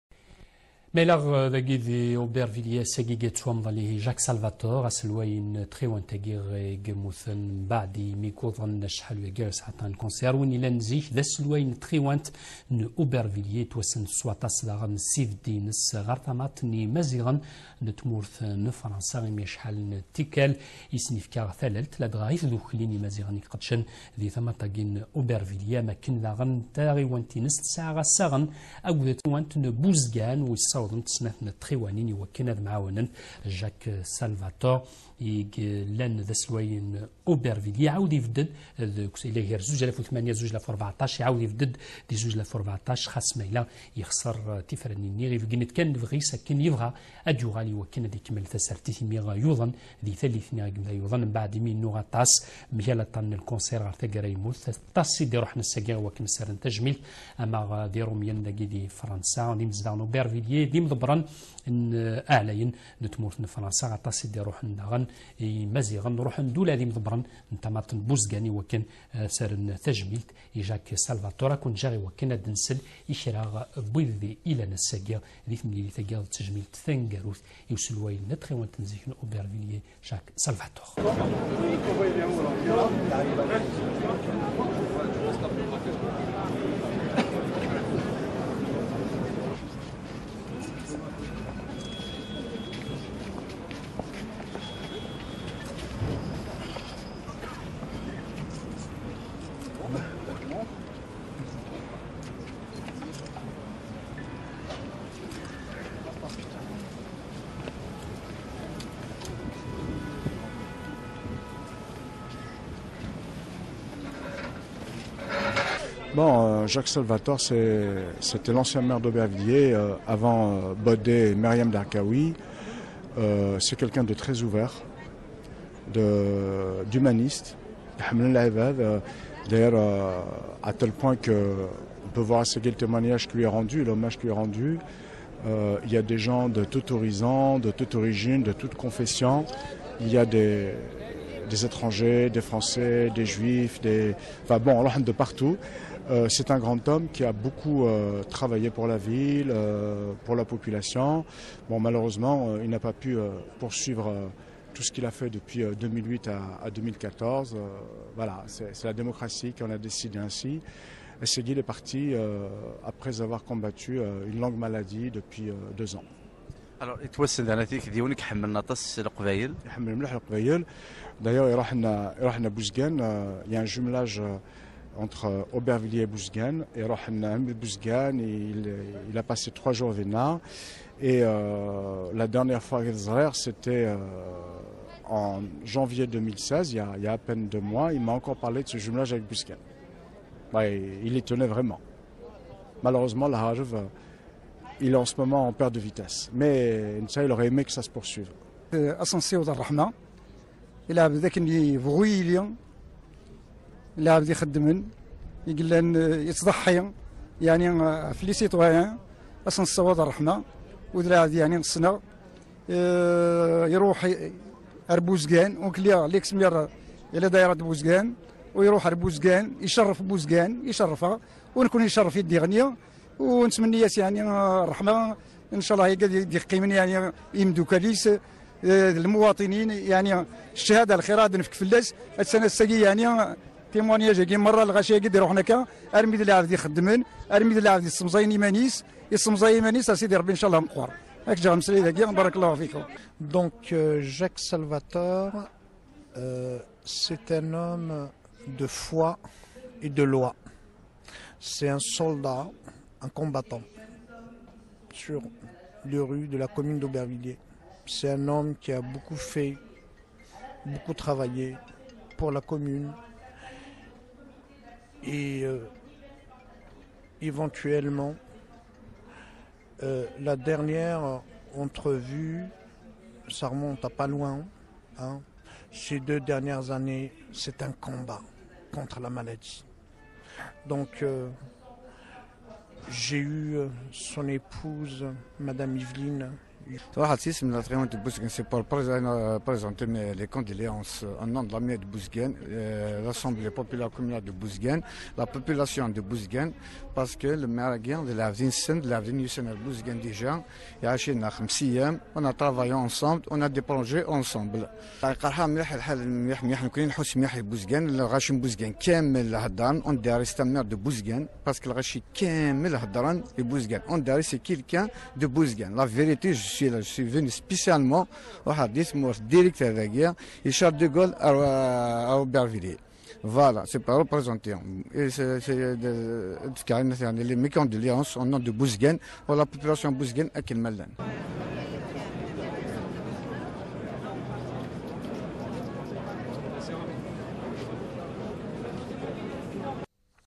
Jacques SALVATOR "Dieu lui fasse pénitence"
La ville de BOUZEGANE et la Kabylie perdent un ami hors du commun, des représentants de la dite ville Kabyle ont été dépêchés pour rendre un dernier hommage et assister à la mise en terre d’un ami de marque que le ciel rappelle à lui endeuillant deux commune celle d’Aubervilliers et de BOUZEGANE que le défunt tenait à jumeler ;témoignant ainsi d’une amitié noble existant entre les populations de ces deux communes et de l’amitié qui prévaut entre la Kabylie et ce peuple profond de France . A cet homme qui nous quitte et que nous espérons restera un modèle et un symbole de l’amour désintéressé  qui peux rapprocher deux peuples nous souhaitons qu’il soit l’hôte des paradis  les plus éthérés et que Dieu dans la hiérarchie de ses invités le placera à sa plus belle enseigne